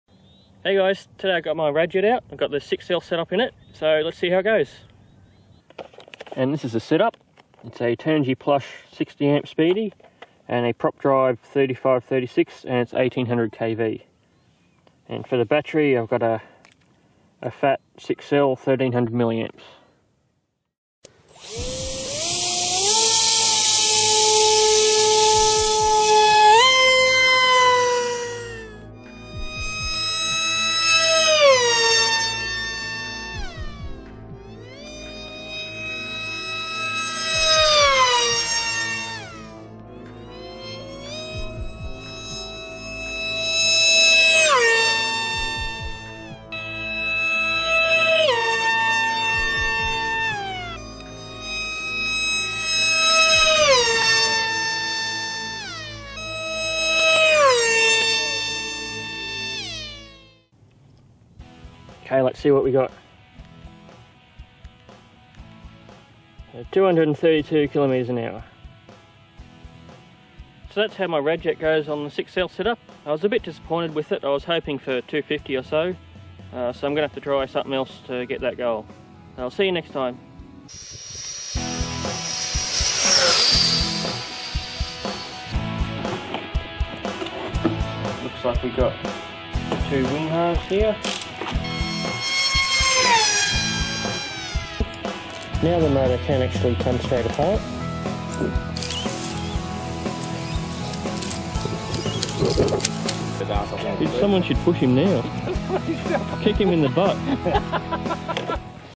RadJet 800 6s 1200 watts (VERY FAST)
The first speed runs of the new 6s setup.
NTM Prop Drive Series 35-36A 1800Kv / 875w